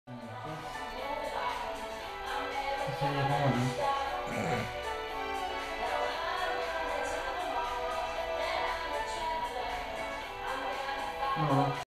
Basic Push ups
Başlangıç için uygun bir şınav egzersizi
dirsekler yana açılırken ellerin arasına göğüs kafesinizi denk getirmeye çalışın
3 x Max. tekrar